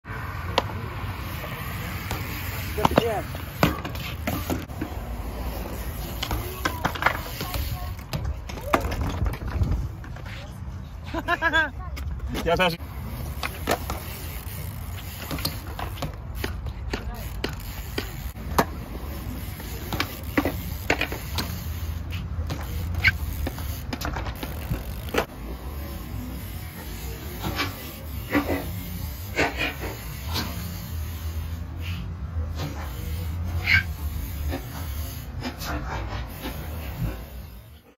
MTB: 360 manual 180
Давно хотел сделать этот трюк на мтб, да, по сути ничего особенного по нынешним меркам, но все равно приятно ☺️, ну и тяжело там было поместиться, все же плаза супер маленькая для таких трамваев 🚋